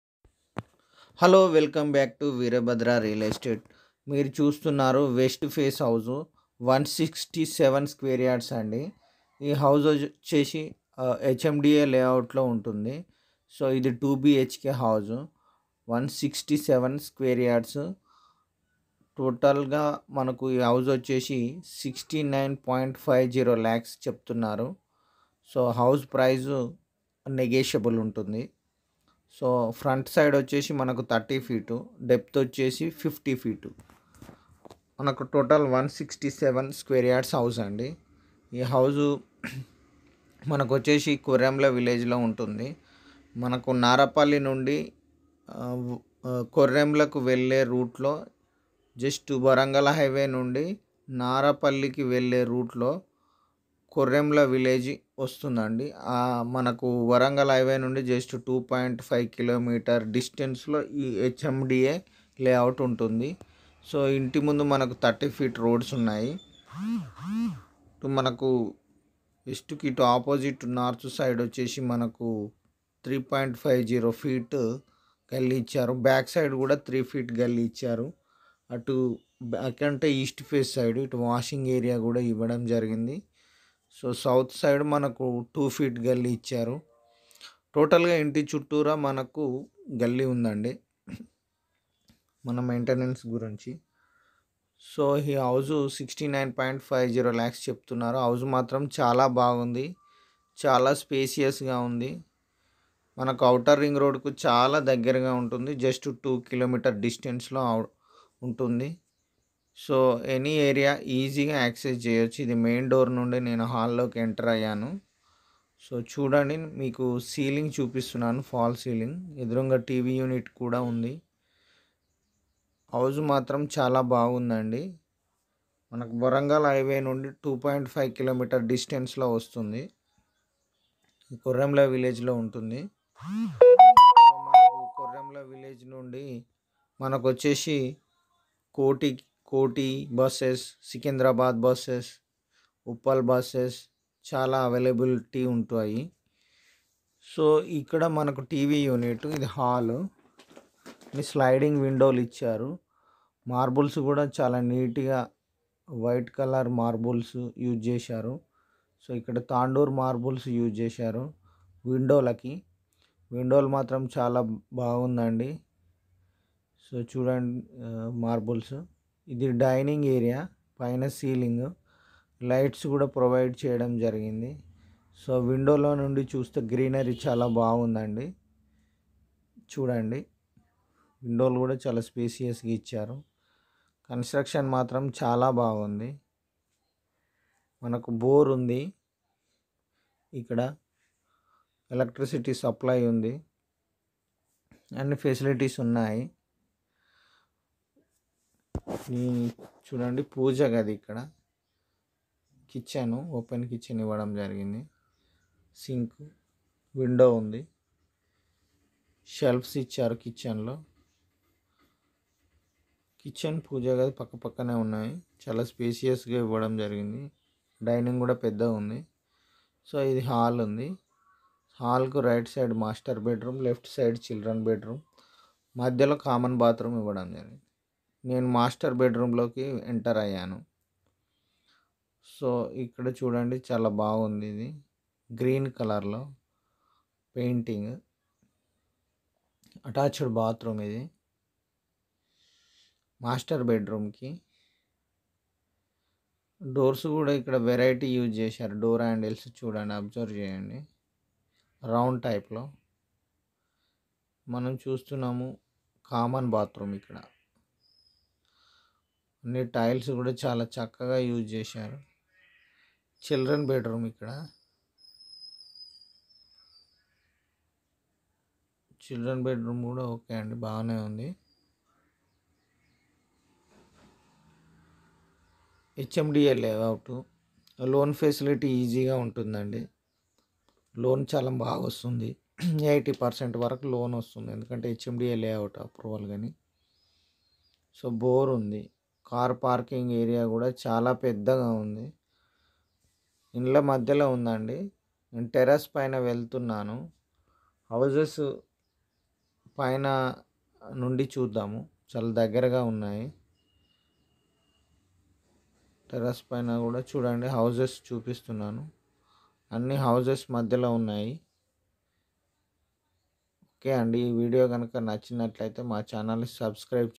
69.50lakhs,167SQ.YARDS West Face 2BHK House,Narapalli Near WGL Highway,8247580577,7989037084
30x50=167sq.yards West Face 2BHK House
Korremula Villege Narapalli Hyderabad

69.50lakhs Price Negotiable
Loan Facility Available
HMDA Layout
Near 2.5kms from waragal highway

బోర్ water Available
Contact Numbers
Mr Ravinder and Mr Krishna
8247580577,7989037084